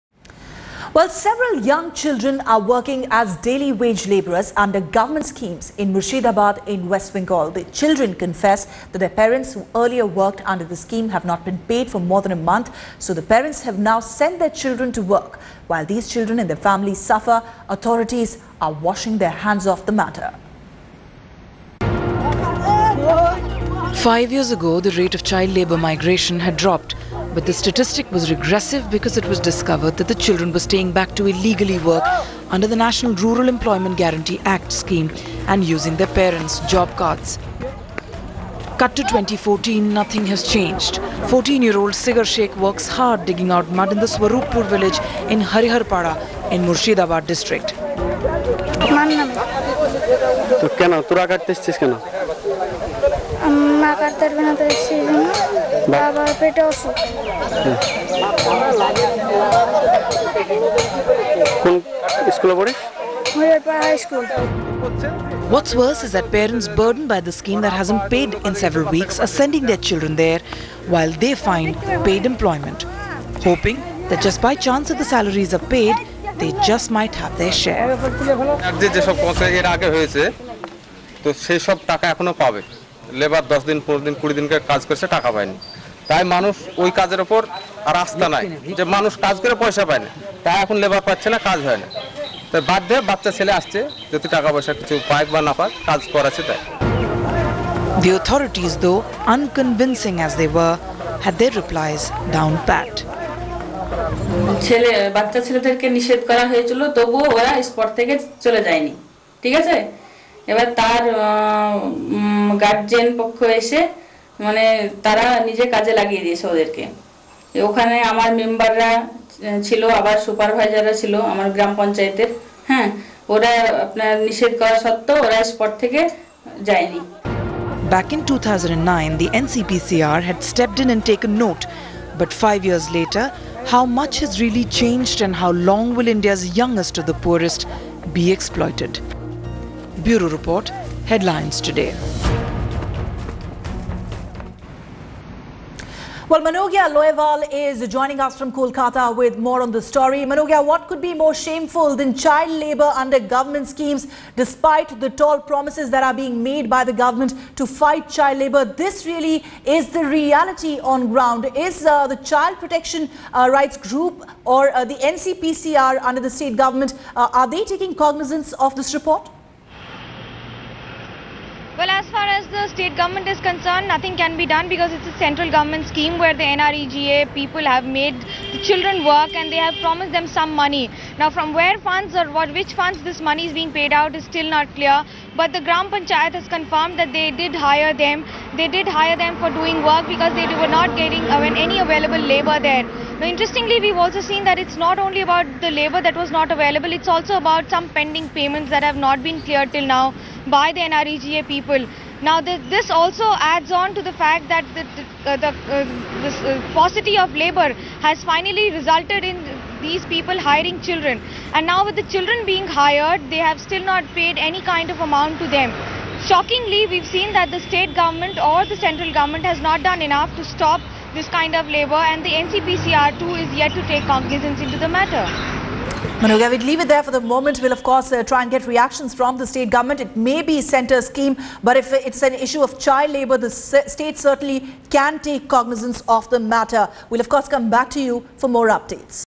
Murshidabad, WB: Employing child labour under job scheme?
Children are using their parents' MNREGA cards and working in Murshidabad, West Bengal and the officials turn a blind eye and wash their hands off the matter. The parents are indulging in this practice because they have not been payed for the last one month and hence need to spend their time working where they will actually be paid, but also don't want to lose MNREGA income in case the people managing the scheme finally figure out the cash to pay the wages.